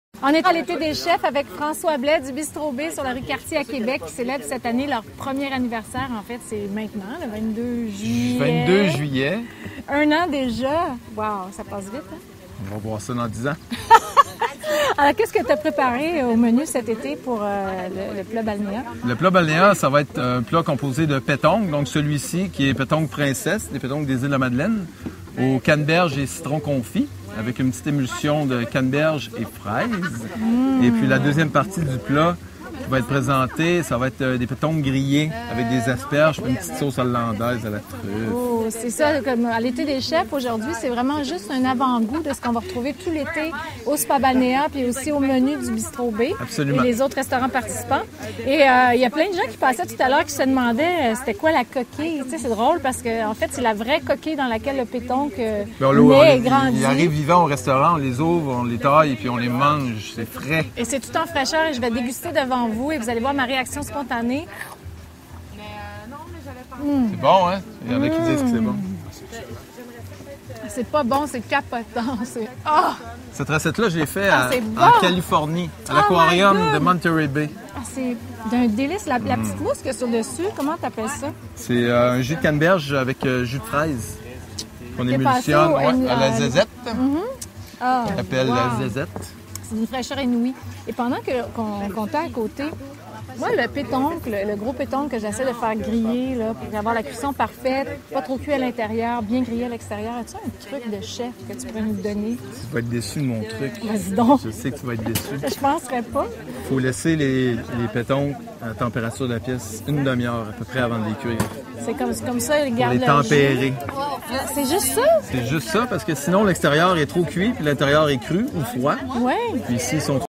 Été des chefs - Francois Blais
Le chef-propriétaire du Bistro B de Québec, François Blais, présentera sa recette de pétoncles Princesse à la purée de fraises et à l'espuma de canneberges au Spa Balnéa. Un régal à déguster également tout l'été, en version duo, à son enseigne de l'avenue Cartier.